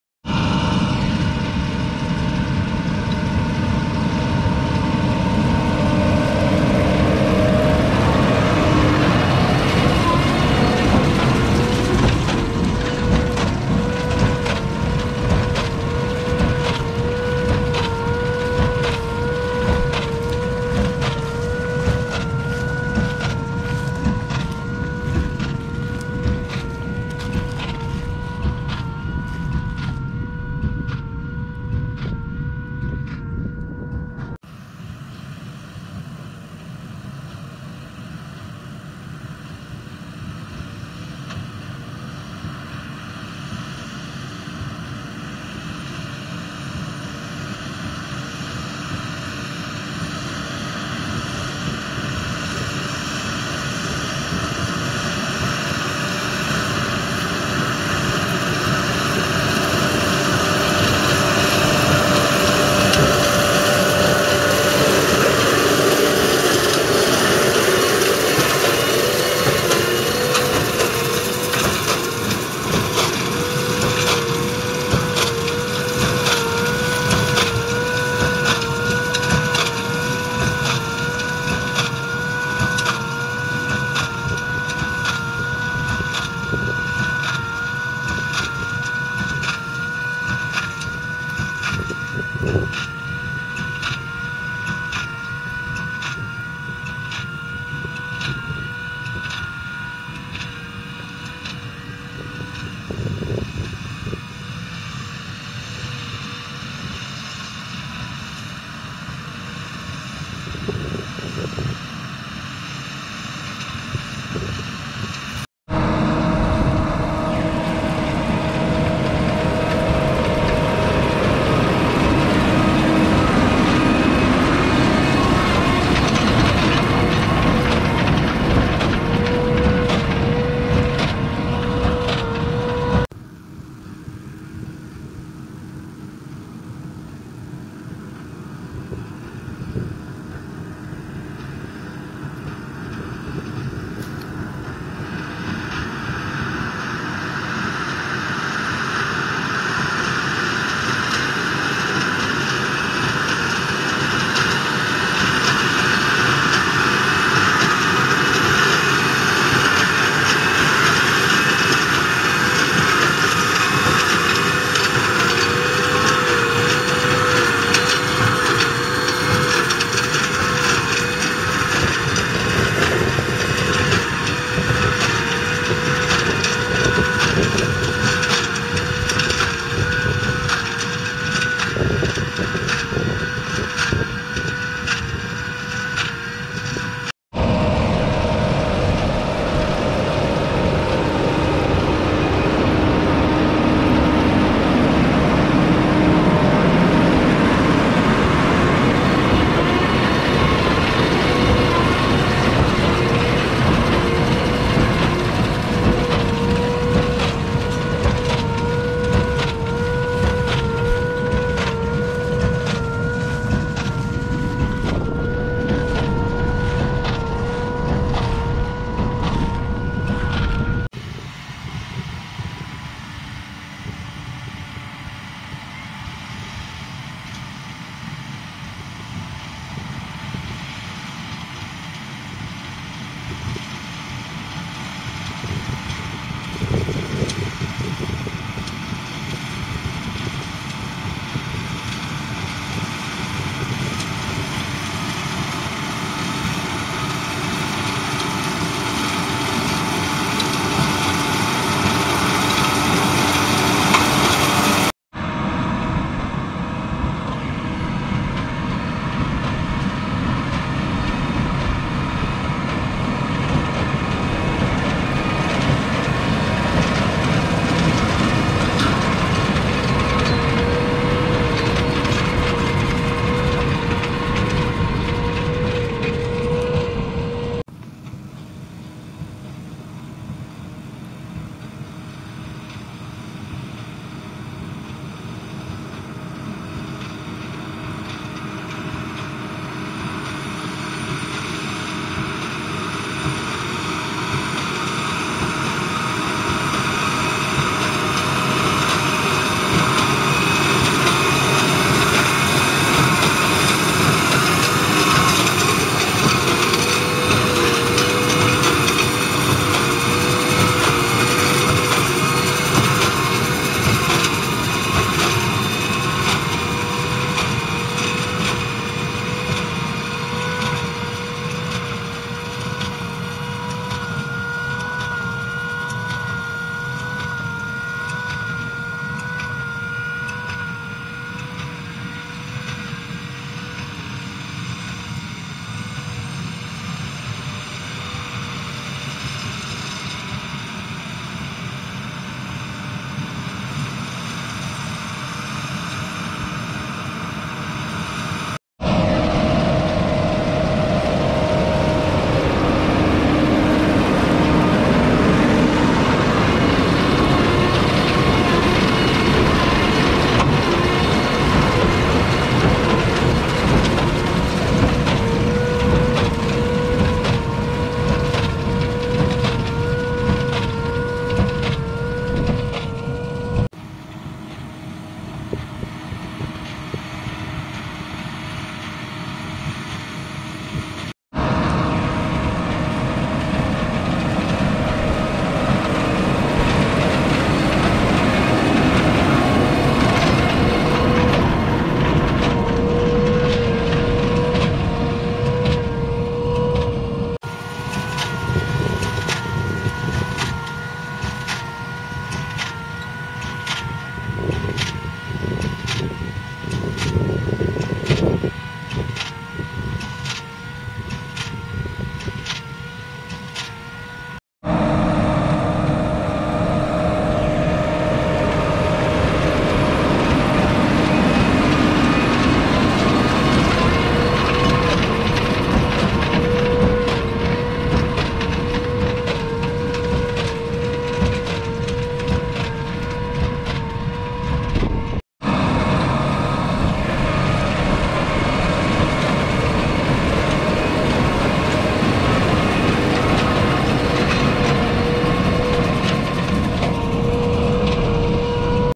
2019 Case IH Puma 200 CVX 6.7 Litre 6-Cyl Diesel Tractor (200/245HP) with Claas Quadrant Bailer
2019 Case IH Puma 200 CVX 6.7 Litre 6-Cyl Diesel Tractor (200/245HP)
Claas Quadrant 5200 Large Square Bailer

Special Thanks to John Watson & Sons Agricultural Contractors, Studley, Warwickshire. England